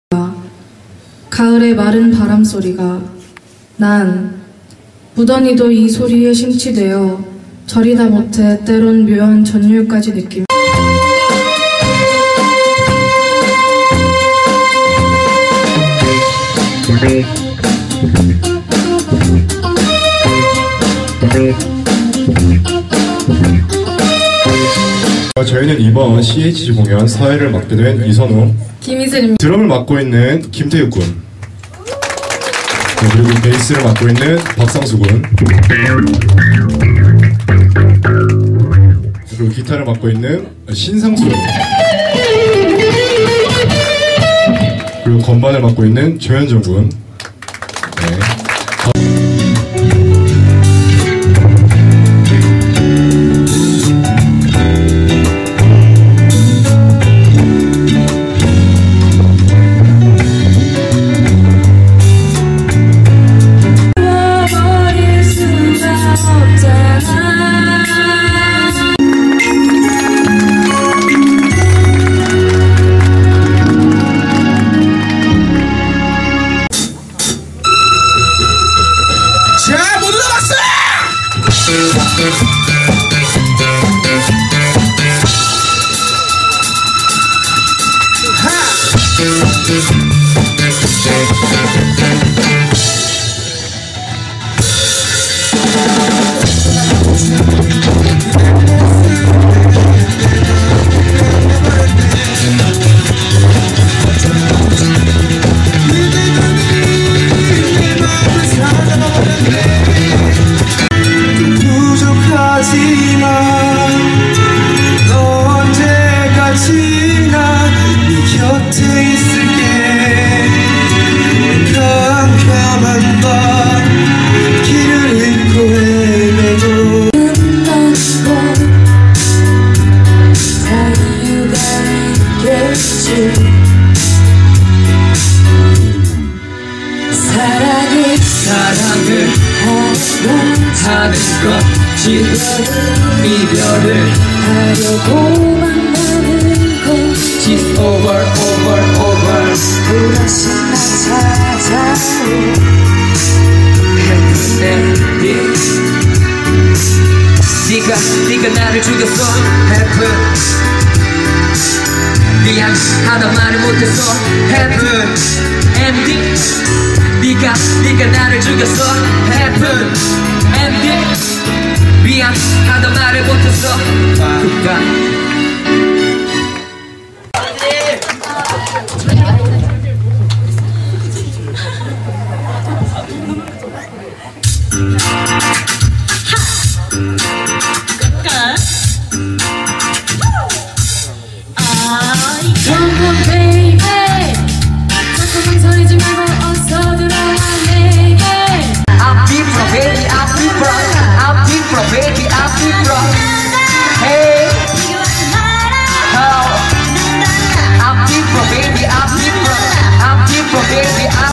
[청주최현군실용음악학원] 2014 송년음악회 (1)
2014년 12월 20일, 청주 최현군실용음악학원에서  송년음악회를 열었습니다.

-keyword-
청주실용음악학원,청주보컬학원,청주기타학원,청주베이스­학원,청주드럼학원,청­주작곡학원,청주미디학원,청주피아노학원,청주재즈피아노학­원,최현군실용음악학­원